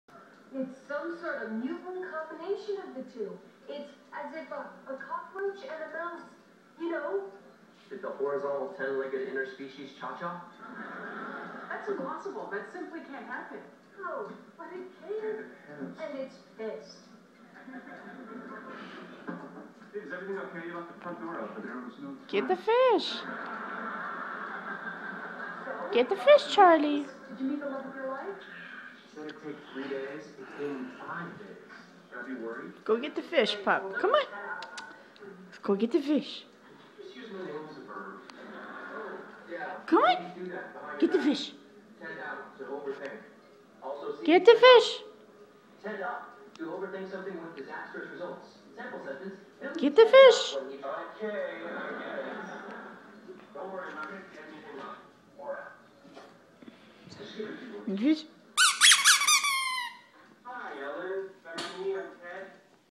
Help Mom! The yellow broom has my toy.
My dog is very afraid of brooms. Unfortunately I end up playing fetch with myself if a toy ever goes close to a broom.